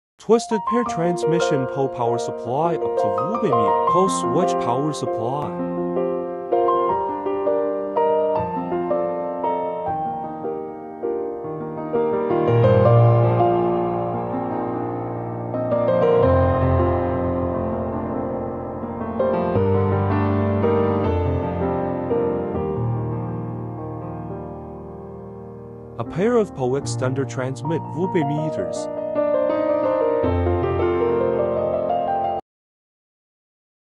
Power over twisted pair 2-wire cable Ethernet repeater with PoE+ 500m PoE extender
The video instruct how to do simulation test and connection. The produce is EA512P 100Mbps long distance 500m PoE IP extender over twisted pair 2-wire telephone cable.
By used in pair,the extender can reach 100+500+100 meters. There are two ends - Slave and Master. Slave end is powered by PoE switch, then Master end to feed any IP CCTV Camera.
When there is no PoE switch available,the pair of extender can be with one 52V/1.25W external power adapter.

Besides 2-wire cable,there can be also cat6 cable via copper RJ45 and coaxial cable via BNC interface.